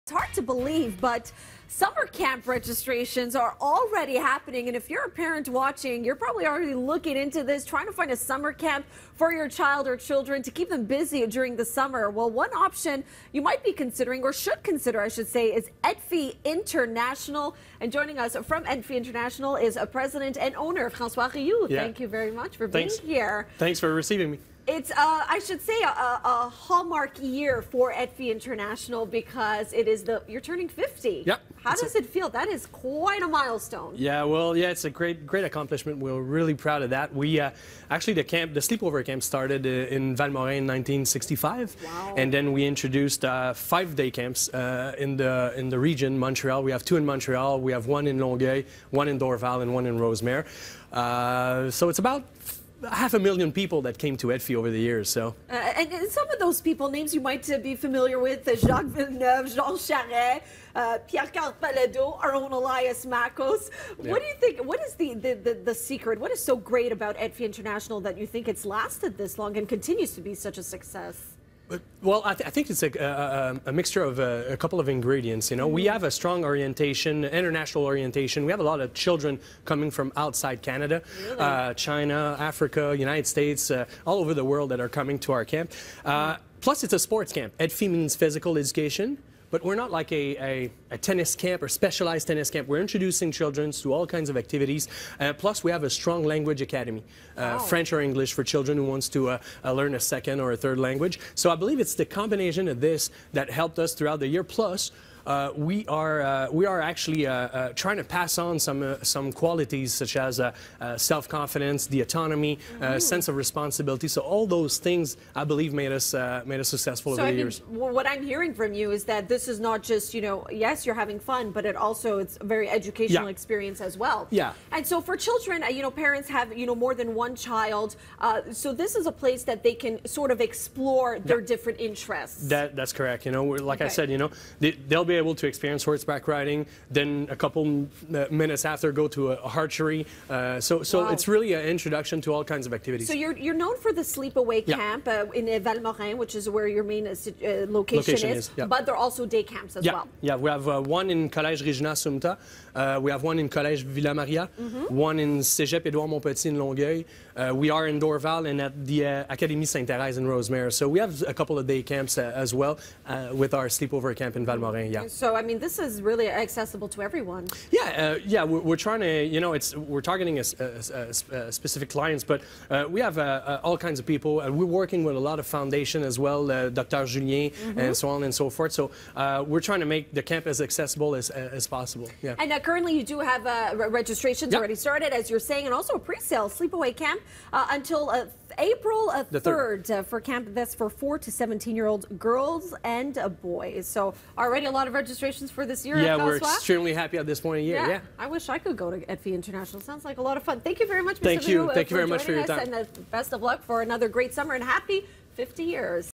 Quebec summer camp celebrates milestone
Spring is right upon us, and inscriptions for summer camps are happening now! If your kids have very diverse interests, EDPHY International might be the right camp for them.